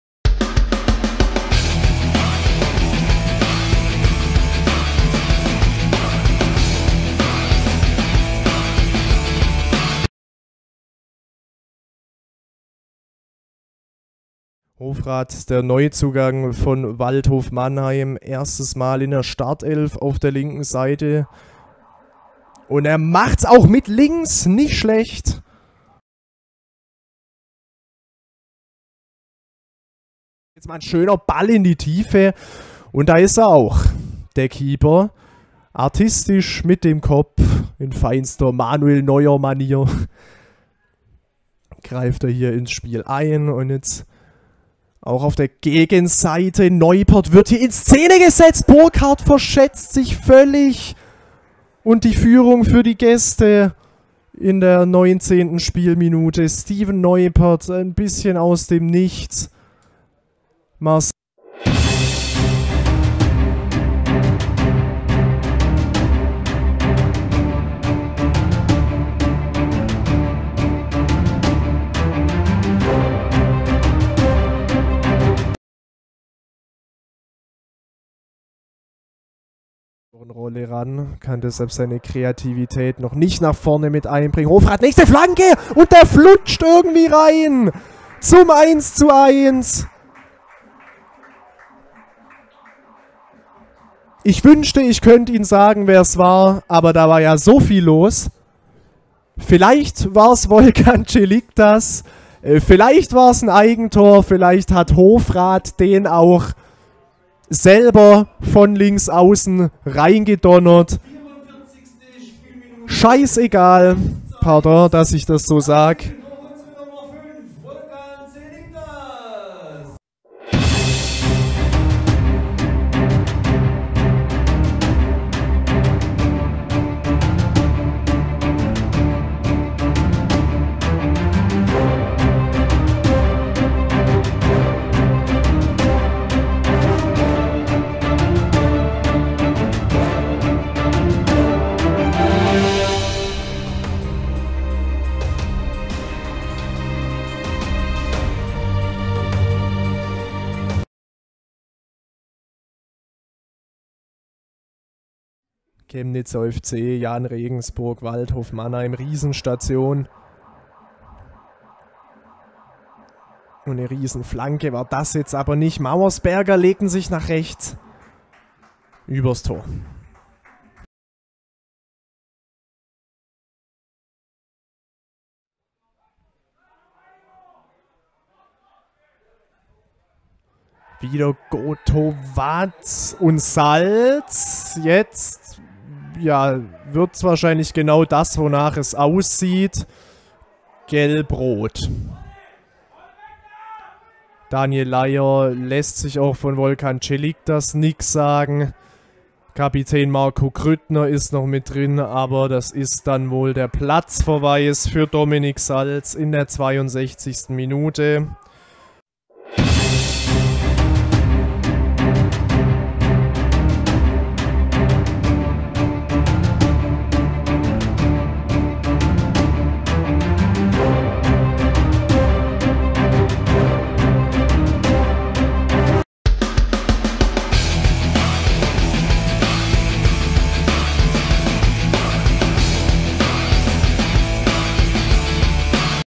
SGV vs Sport-Union Neckarsulm I 11. Spieltag OLBW | 02.10.2021 | SGV-TV I Highlightshow | S. 21-22
Hier die Highlights vom 1:1 gegen die Sport-Union Neckarsulm!